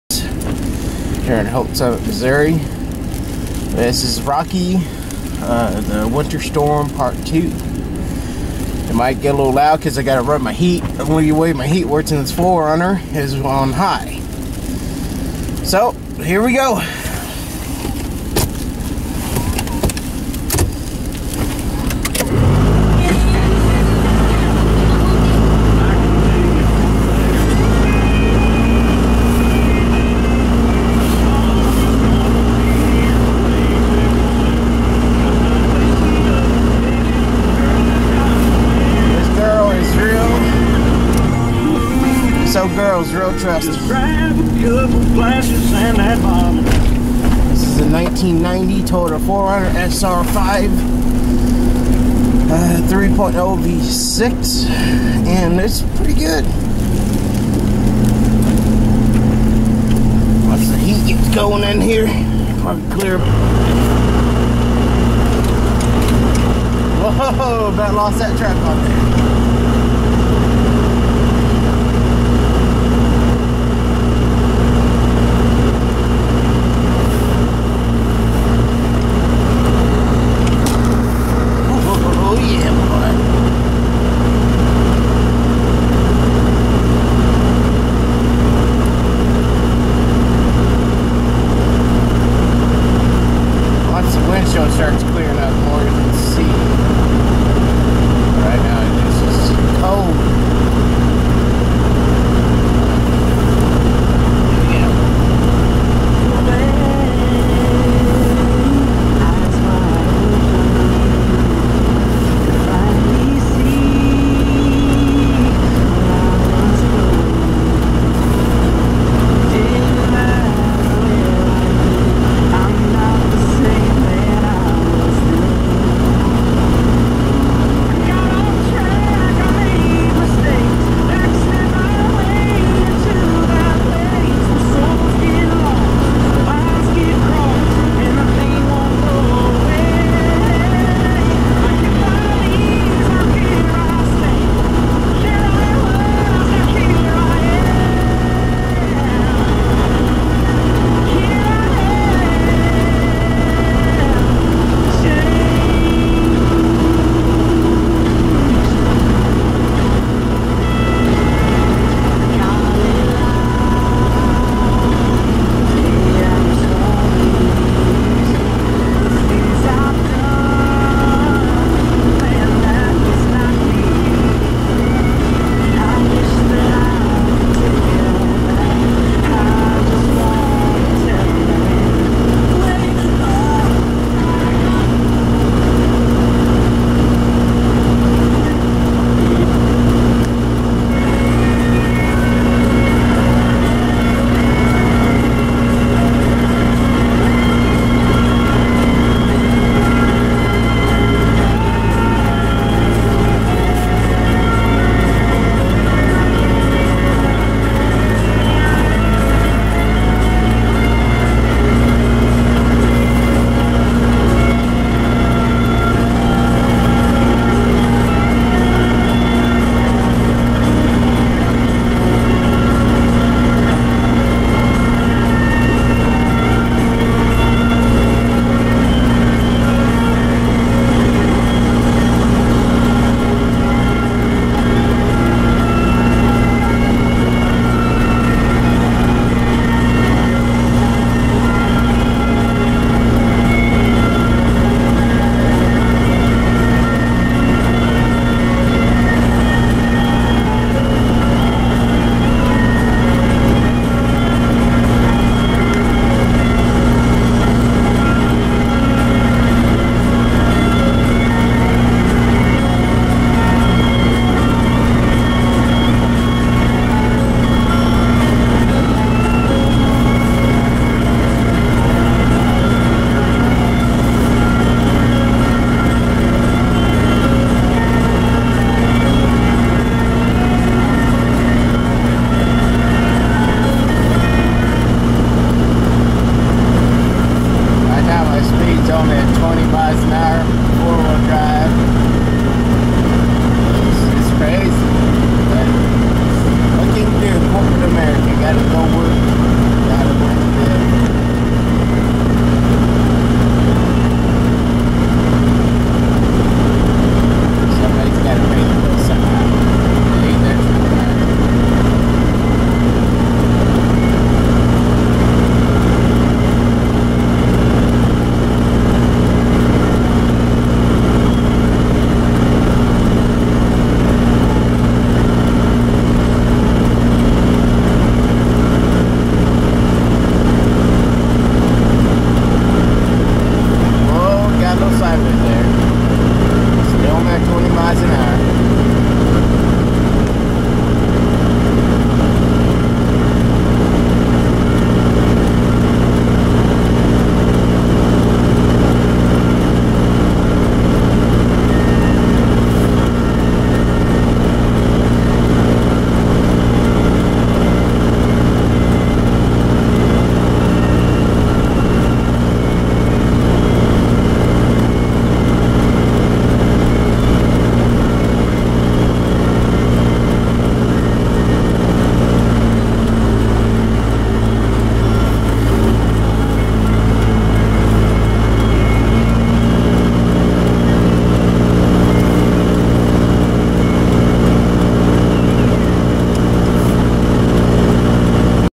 Snow storm Rocky.
Heading to work. In a 1990 Toyota 4RUNNER. Video camera was on a tripod.This was around 415am. Music was playing on the Radio.